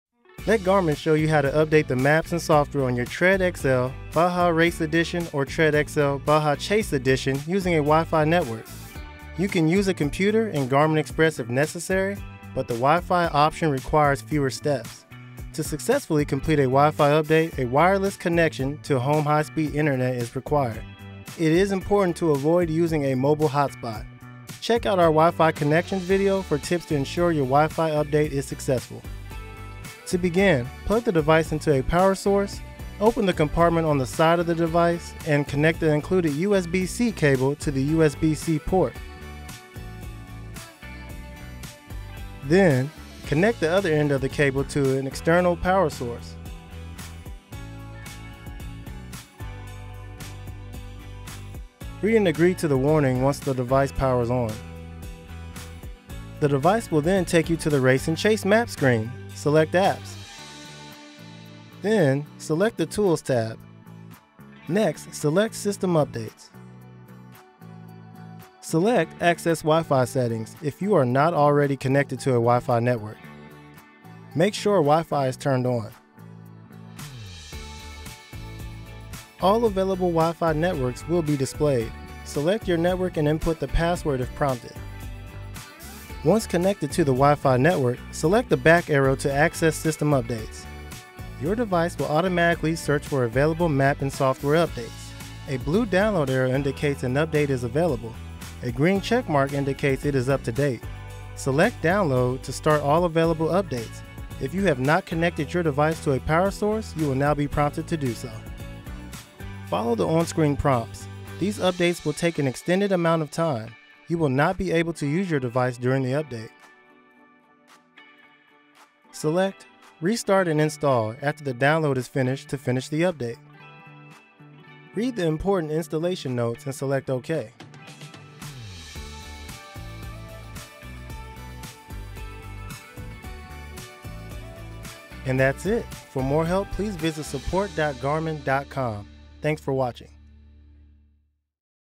Garmin Support | Tread™ XL - Baja Race/Chase Edition | Wi-Fi® Updates
Learn how to set up and use Wi-Fi® to update your Tread™ XL - Baja Race or Baja Chase Edition.

Wi-Fi is a registered trademark of the Wi-Fi Alliance